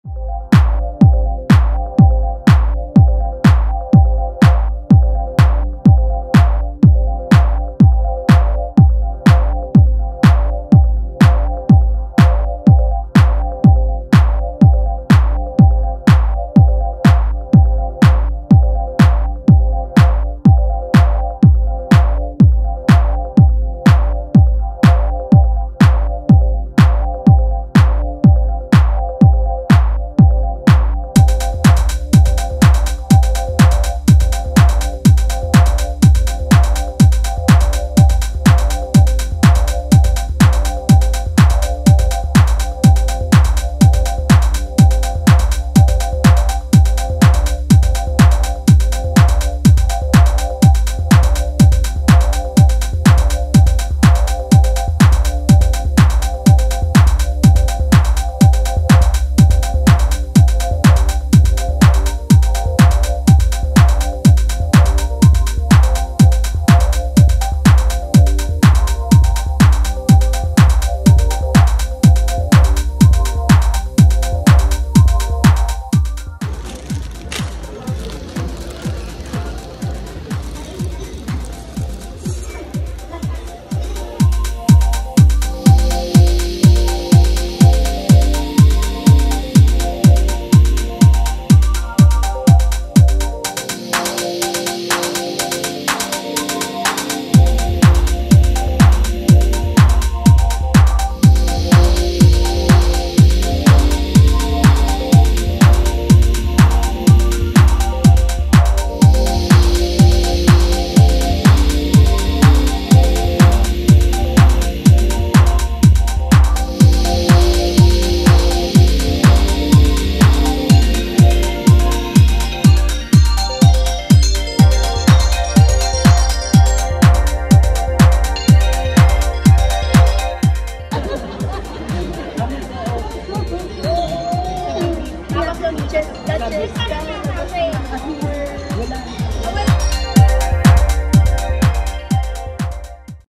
LUNCH BUFFET at CAFE IN THE PARK
LUNCH AND DINNER BUFFET at CAFE IN THE PARK is open once again.  As they level up the dining game experience, they have recently introduced their Churrasco-style buffet where everyone can enjoy unlimited portions of grilled meats together with your old-time favorite Century Park Hotel dishes.